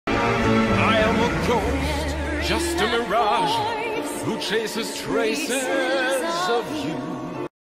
I know those eyes wip 1
Oh boy it’s so hard lip syncing in firealpaca but oh well I’ll try to fix the timing